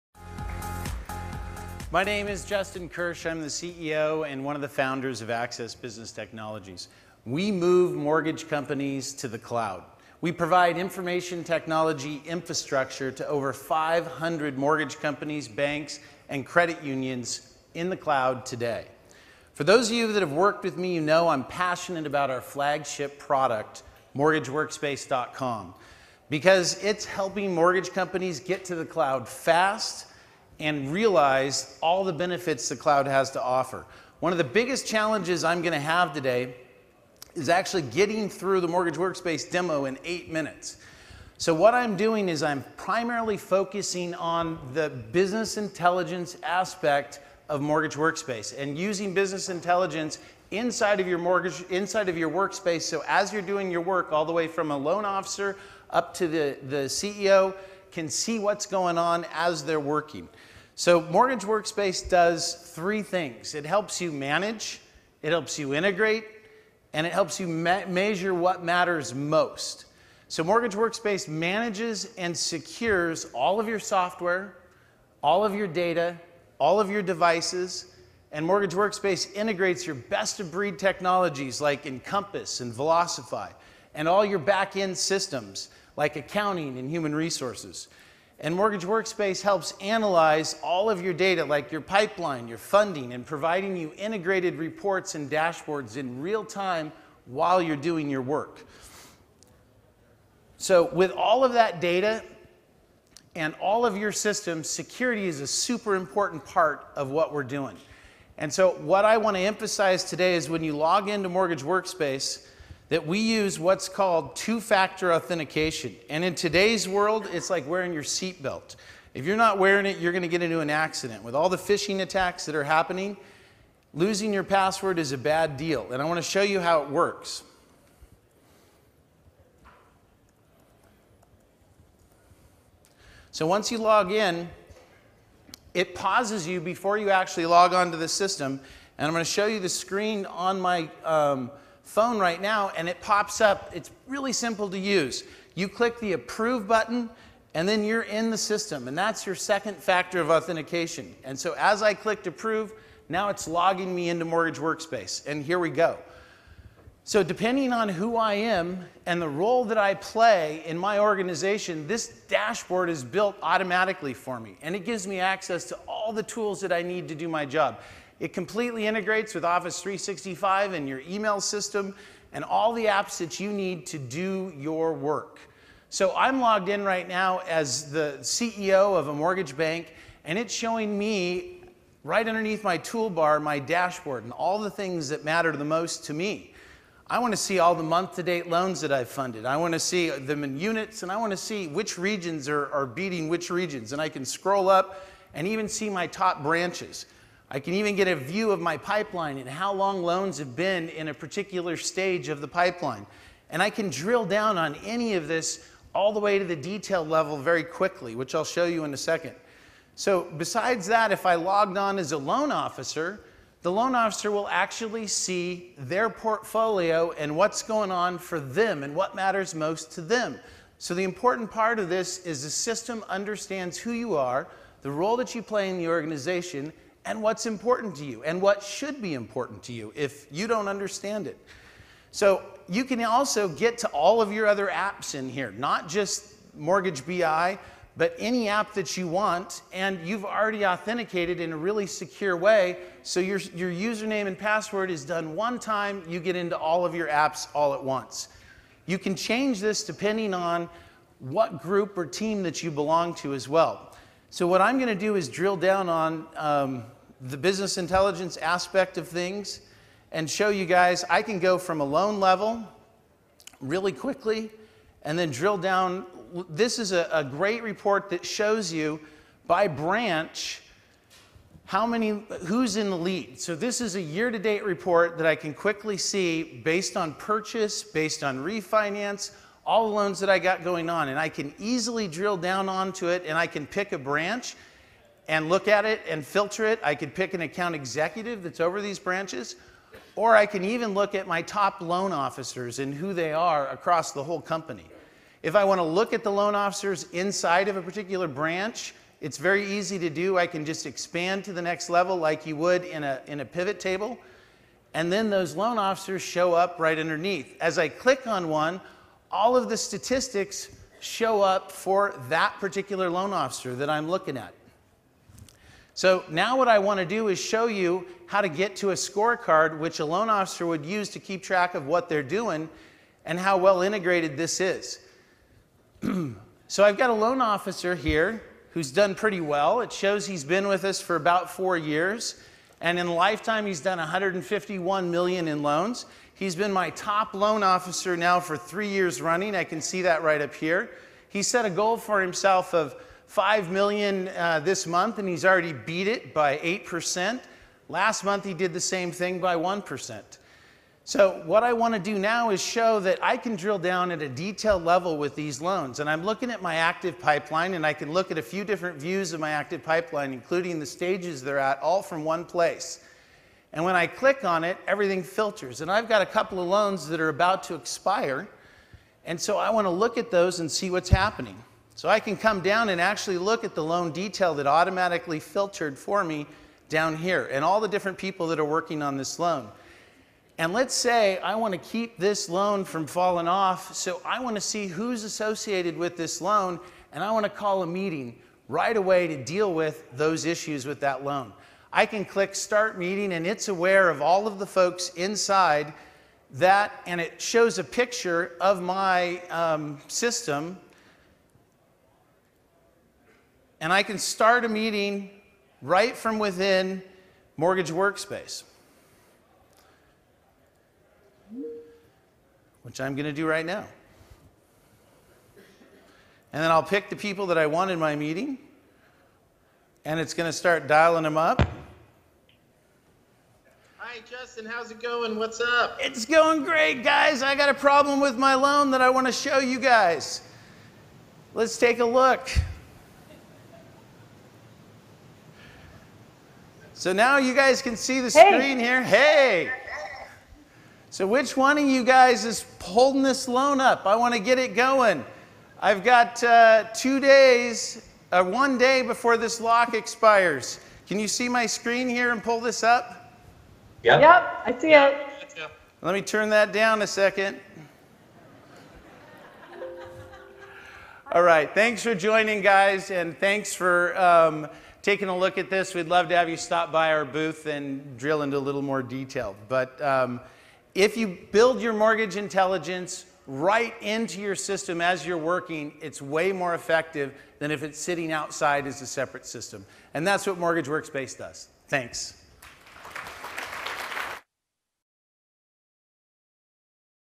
Digital Mortgage Demo ABT
Justin Kirch, CEO at Access Business Technologies, presents MortgageWorkSpace.